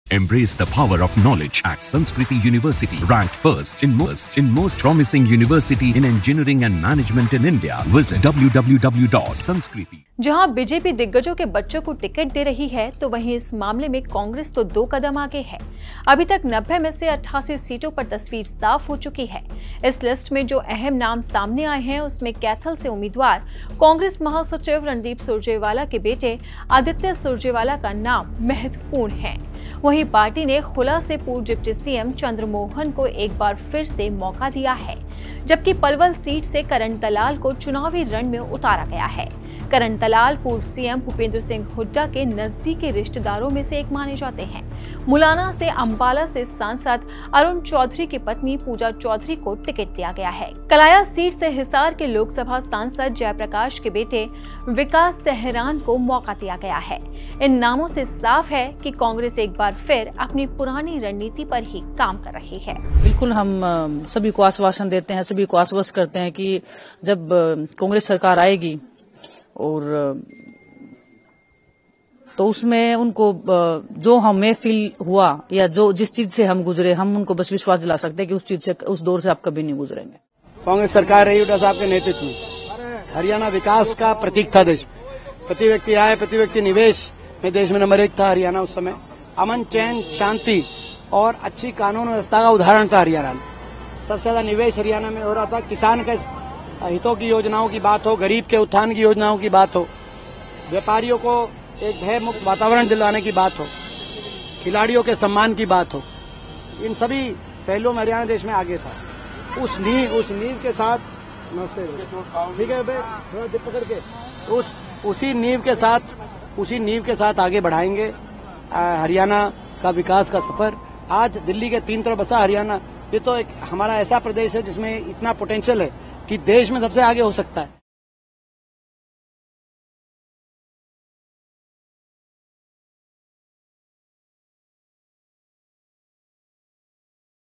Haryana Assembly Election 2024: हरियाणा में क्या है Congress का प्लान | BJP List
Haryana BJP Second candidates List: हरियाणा में विधानसभा चुनाव को अब ज्यादा वक्त नहीं बचा है...अगले ही महीने हरियाणा की जनता नई सरकार को चुनने में अपनी अहम भूमिकका अदा करेगी...लेकिन जनता से पहले बारी है राजनीतिक दलों की, कांग्रेस से लेकर बीजेपी जिताऊ उम्मीदवारों पर दांव लगाकर जीत की राह को आसान बनाने की कोशिश कर रहे हैं...लेकिन इस बार टिकट बंटवारे में सबसे ज्यादा बोलबाला है 'परिवारवाद' का...कांग्रेस के साथ-साथ बीजेपी ने भी इस बार नेताओं के बेटे-बेटियों पर दांव लगाया है...यानी जीत का ताज पहनने के लिए दोनों ही दल एक जैसी रणनीति पर काम कर रहे हैं...तो किस पार्टी ने  किसे दिया टिकट...देखते हैं इस रिपोर्ट में -